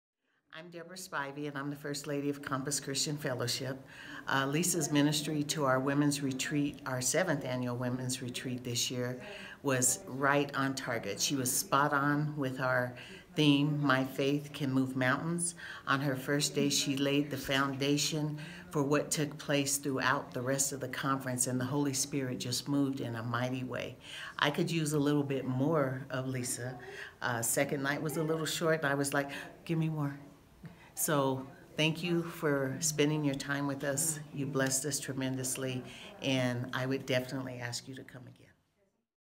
Speaker Promotion 2
Debra Spivey of Compassions Christian Fellowship gives a testimony of her experience with Lisa D. Edmonds as a conference/retreat speaker